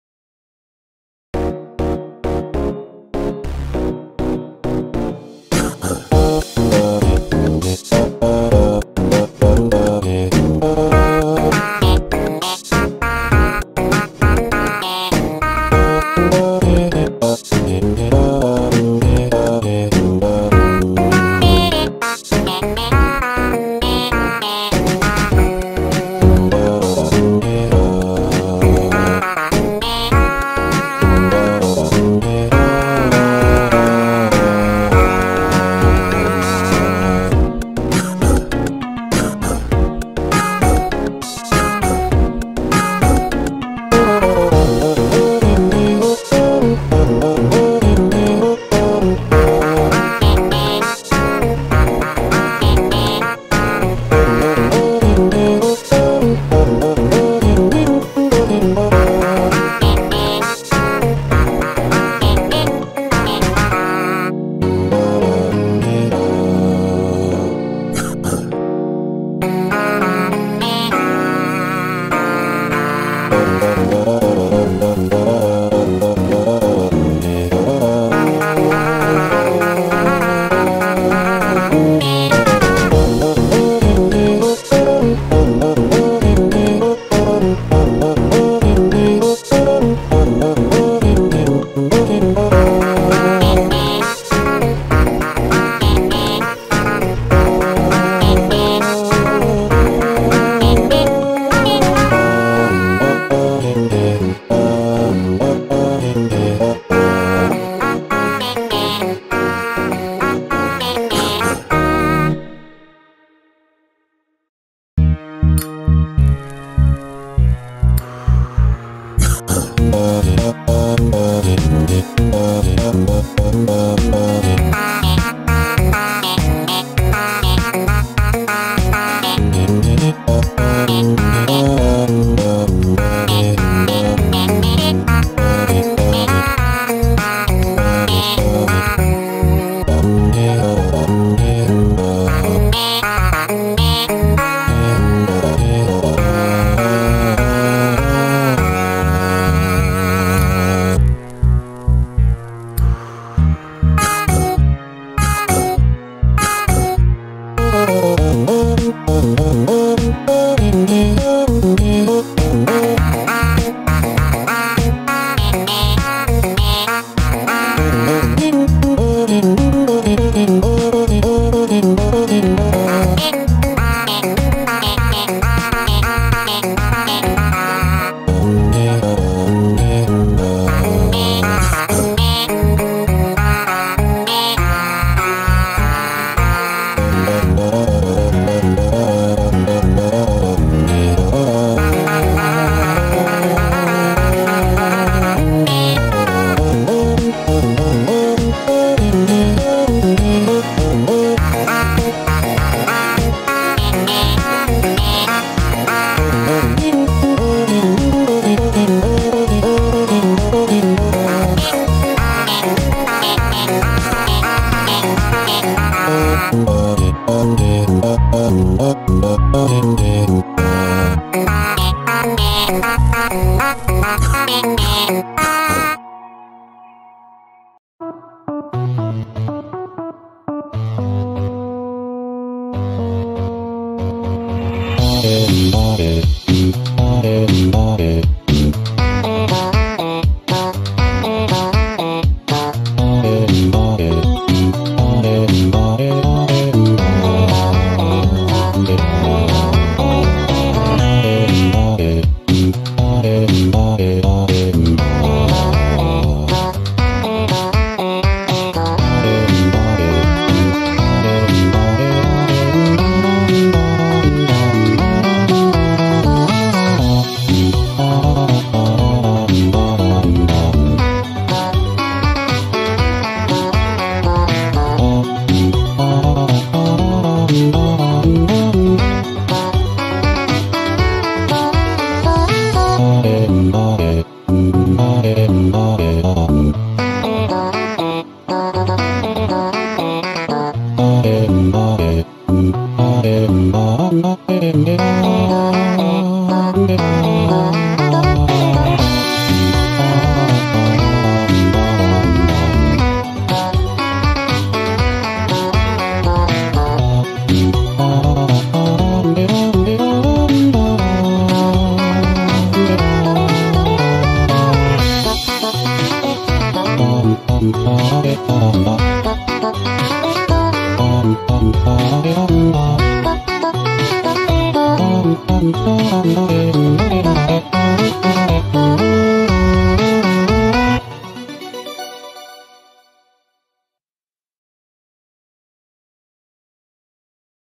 Drawing Garcello (Friday Night Funkin)
Alrighty the next Character is finally finished I hope you guys like the drawing and no worries this might be the last Friday Night Funkin Character I might do in a while but next you might know who
the one that lurks in peoples Nightmare’s